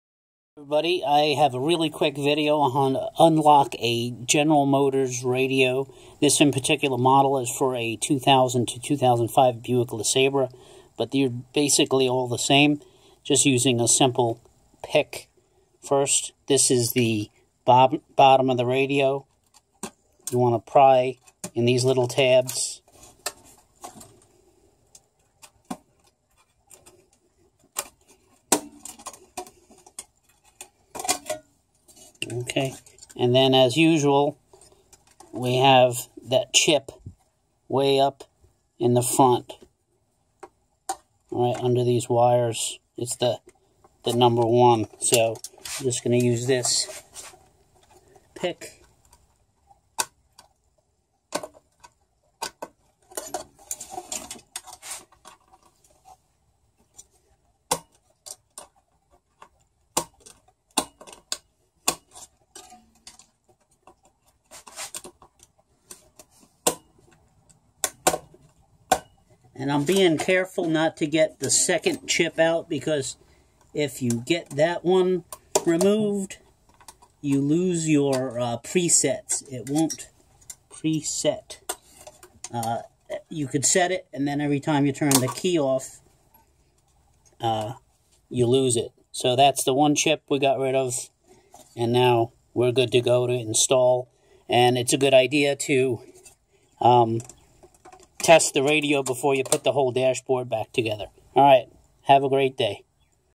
Quickly unlock GM Theftlock radio.
One tool used to unlock a used GM radio from 2000 to 2008